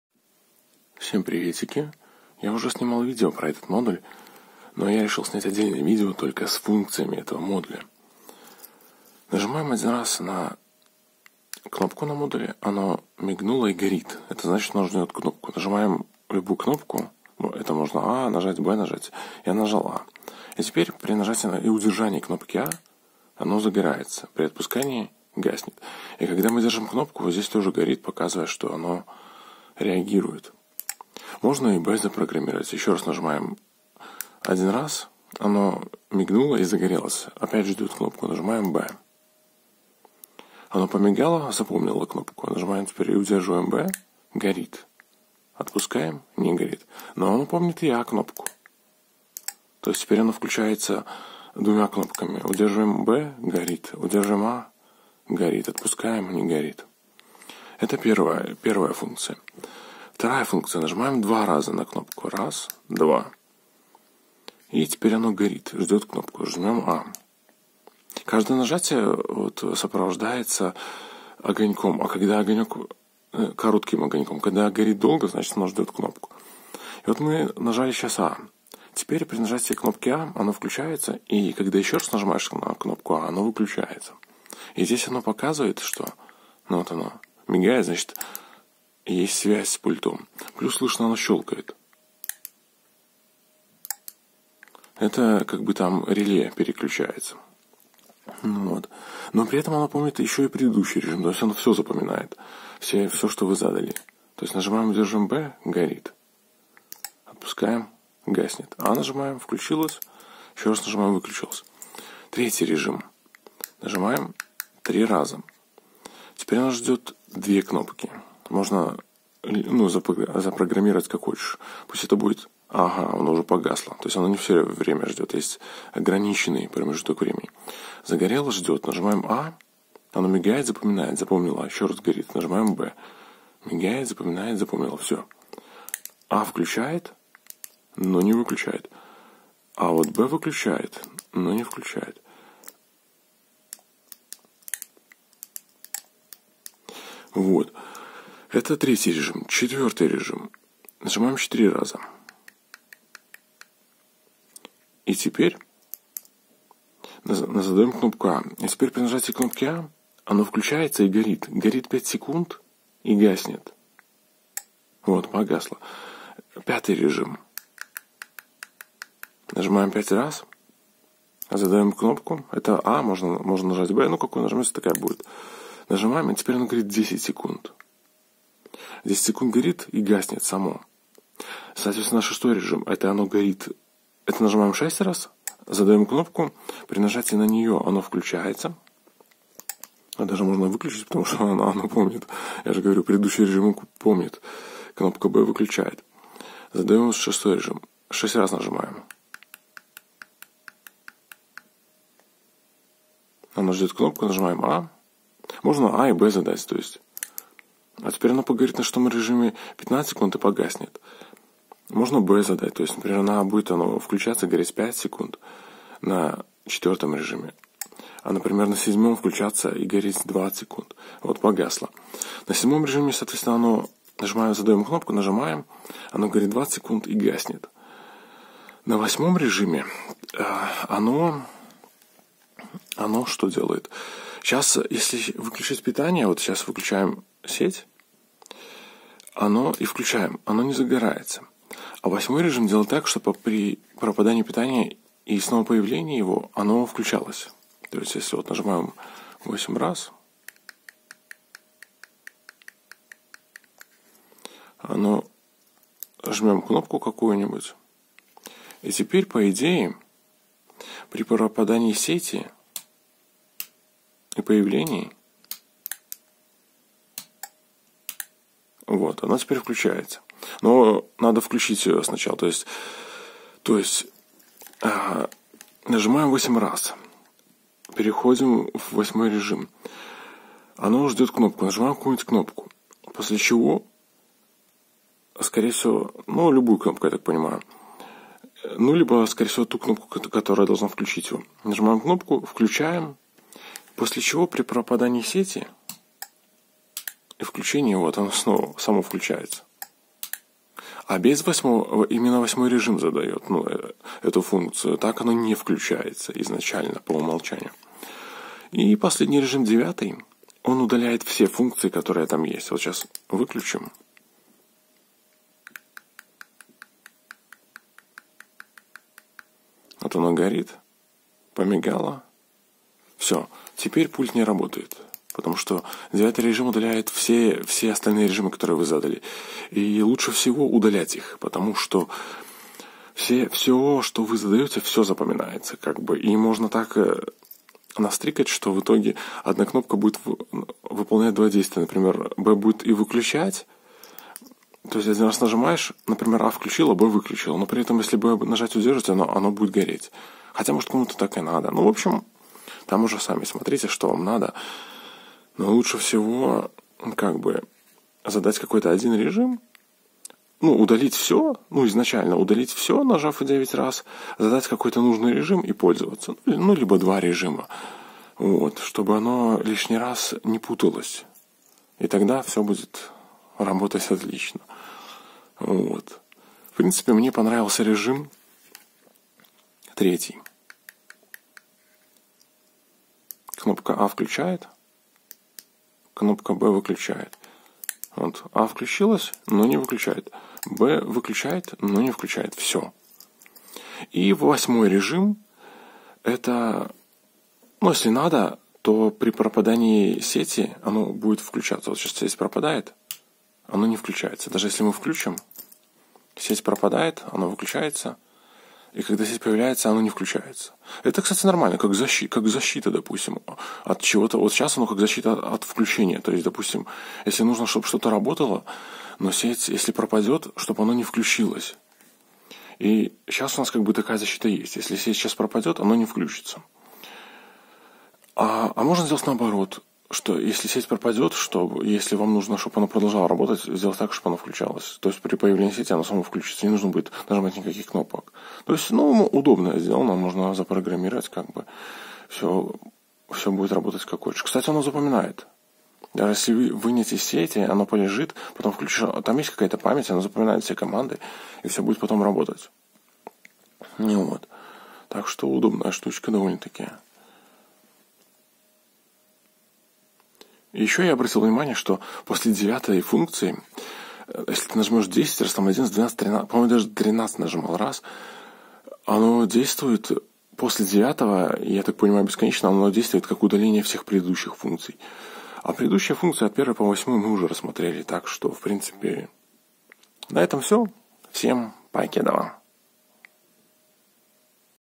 Беспроводной переключатель, радиочастотный передатчик - Обзор функций
Но там я показывал и рассказывал ещё и про удлинитель, и про то как я всё это собрал. Ну и в целом полностью всё рассказал про переключатель.
А тут я решил сделать отдельное видео только про функции этого переключателя. Чтобы если кому-то нужно быдет разобраться с функциями, то он мог бы быстро это сделать :)